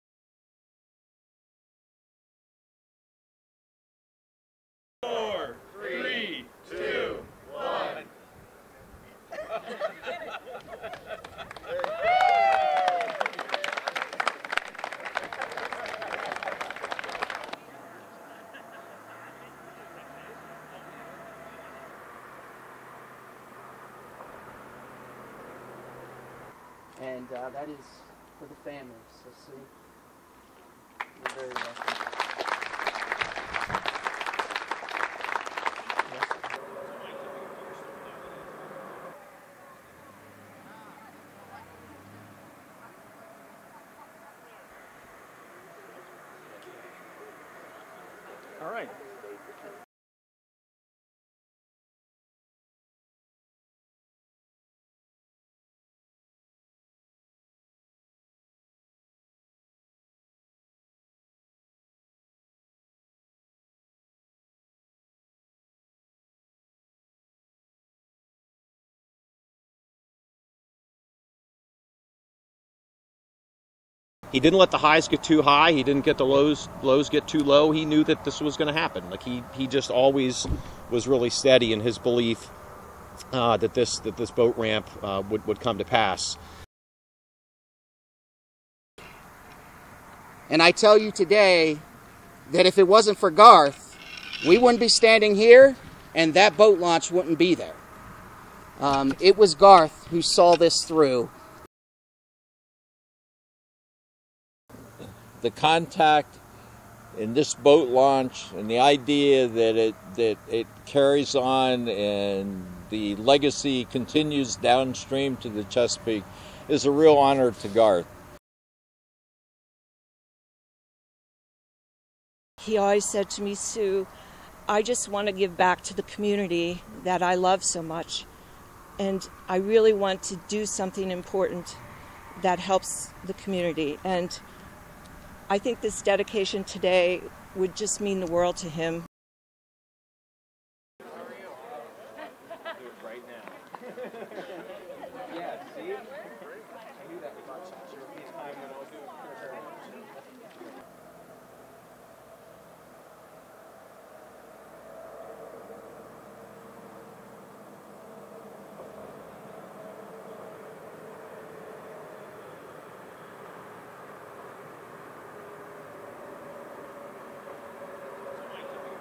Dedication of Rep. Garth Everett Muncy Access Area (B-Roll)
On Friday, October 6, 2023, the Pennsylvania Fish and Boat Commission (PFBC) celebrated the opening of the newly constructed Muncy Boat Access in Muncy Creek Township, Lycoming County.  The access along the West Branch Susquehanna River was dedicated to the memory of the late Representative Garth Everett who died in January of 2023 after a lengthy battle with cancer. Everett served in the Pennsylvania House of Representatives for 14 years and advocated for sportsman’s issues in the 84th District and as a member of the Chesapeake Bay Commission.

The former Muncy Access was closed in July 2016 due to unsafe conditions caused by the accumulation of sediment from a nearby creek, which created a sandbar at the old boat launch.  The new boat launch has been constructed safely downstream, with a new access road connected to the existing parking lot.

Representative Everett is credited with securing funds for the project, which cost approximately $356,000.